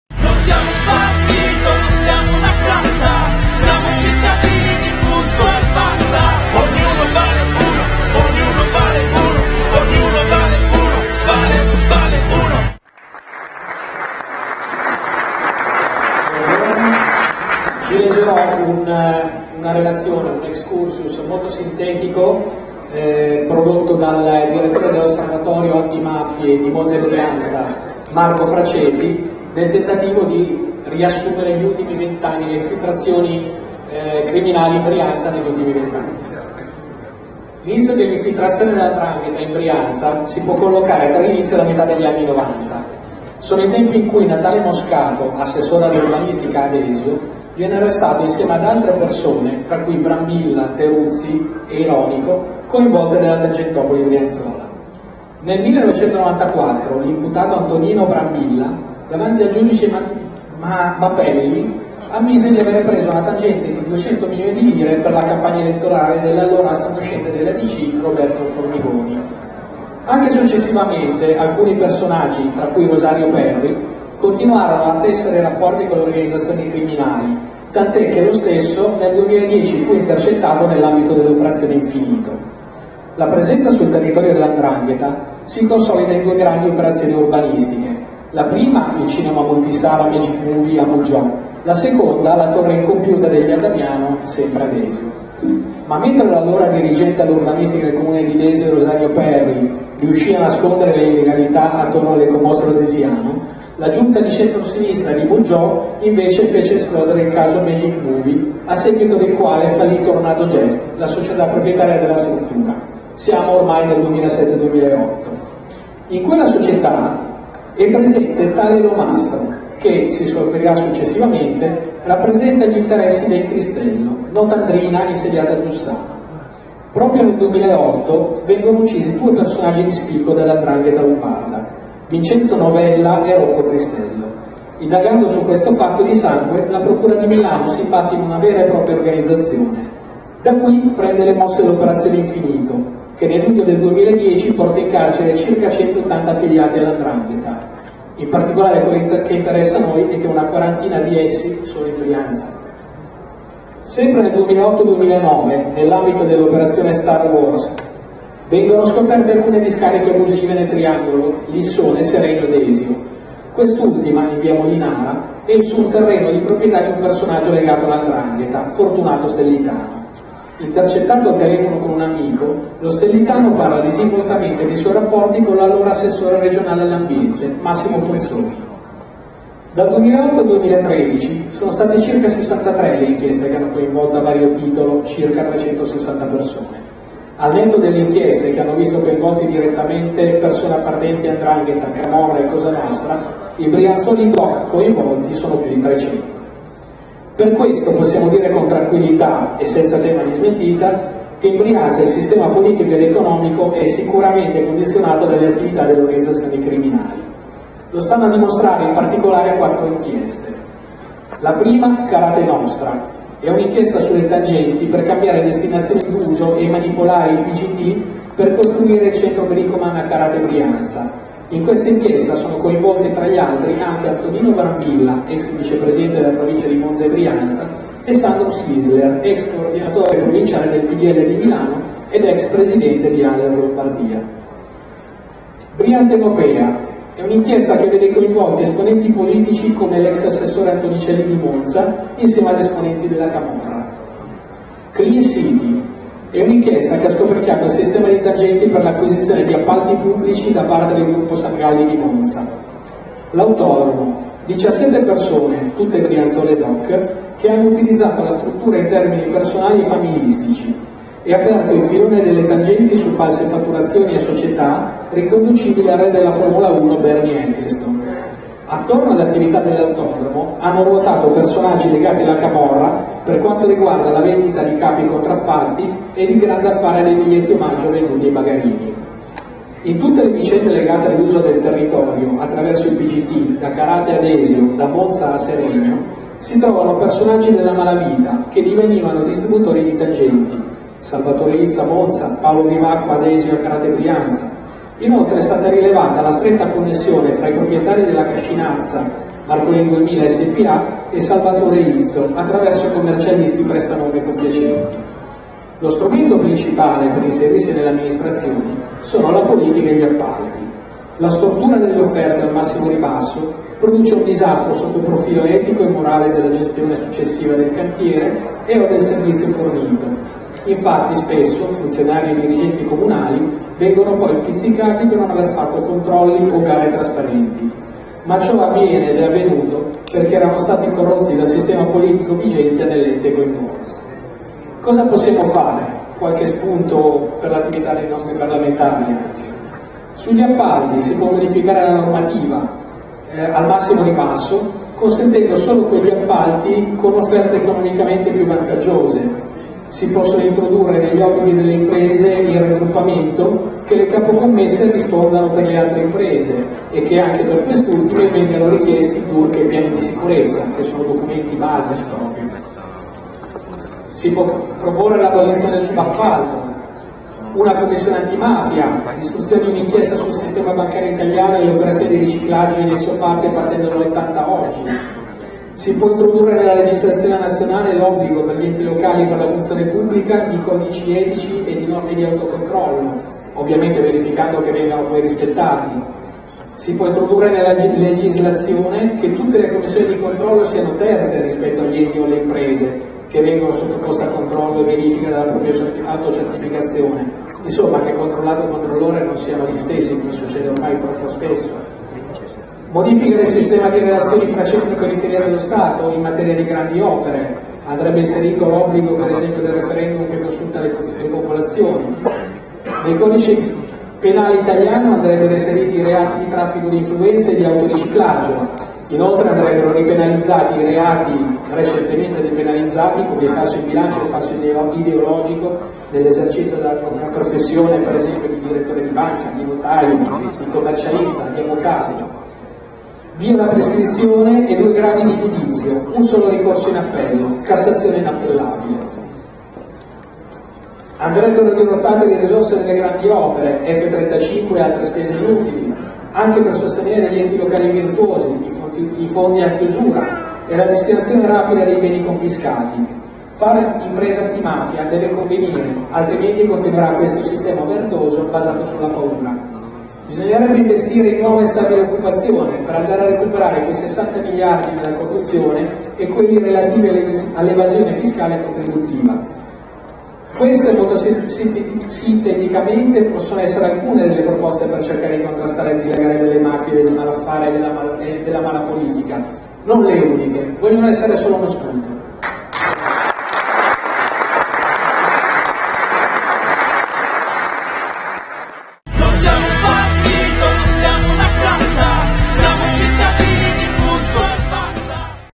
I parlamentari incontrano i cittadini parte 6
Relazione di Massimiliano A. sulle ecomafie in Brianza - 22 febbraio 2014